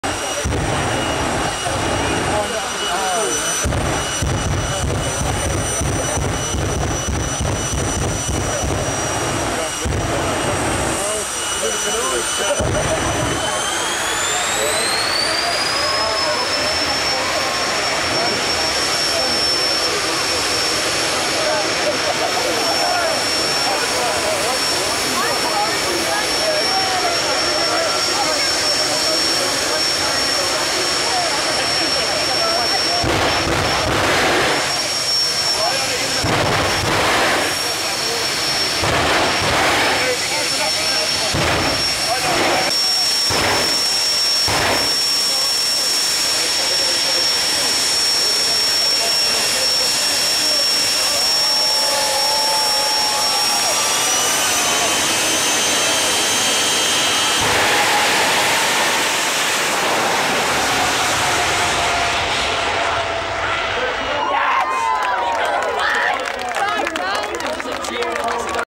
FireForce 3 at Night - Santa Pod European Finals 2014
To close out the Friday night show Martin Hill in the fireforce 3 jet funny car blazed down the quarter mile running a 5 second pass